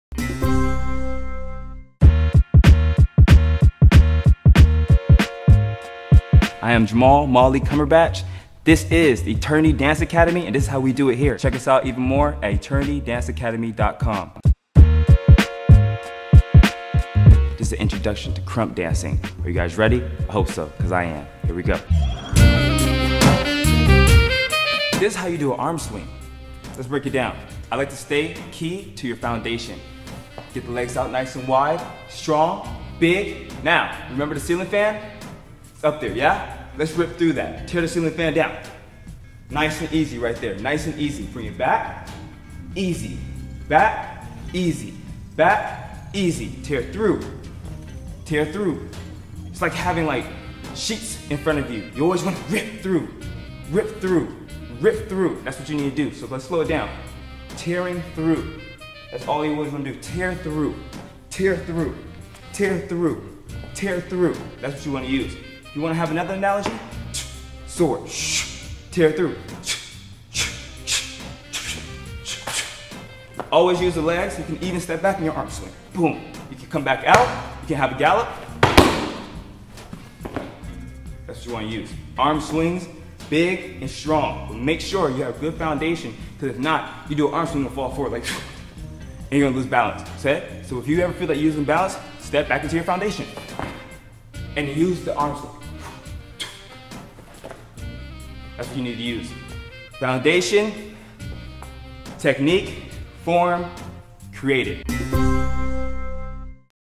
How to Do an Arm Swing | Krumping
I am Jamal Cumberbatch. This is Eternity Dance Academy and this is how we do it here. Check us out even more at Eternitydanceacademy.com
This is an introduction to krump dancing. Are you guys ready? 'Cause I know I am, here we go.. This is how you do an arm swing. Let's break it down. I like to stay key to your foundation. Get the legs out nice and wide. Strong, big. Now remember the ceiling fan? It's up there, yeah? Let's rip through that. Tear the ceiling fan down. Nice and easy right there. Nice and easy. Bring it back, easy, back, easy, back, easy. Tear through, tear through. It's like having sheets in front of you. You always want to rip through, rip through, rip through. That's what you need to do. So let's slow it down. Tearing through. That's always what you want to do. Tear through, tear through, tear through. That's what you want to use. You want to have another analogy? Sword. Tear through. Always use the legs. You can even step back in your arms swing. You can come back out. You can have a gallop.That's what you want to use. Arm swings, big and strong. Make sure you have good foundation. Because if not you do an arm swing, you're going to fall forward and you're going to lose balance. So if you ever feel like you're going to lose balance, step back into your foundation and use the arms swing. That's what you need to use. Foundation. Technique. Form. Creative.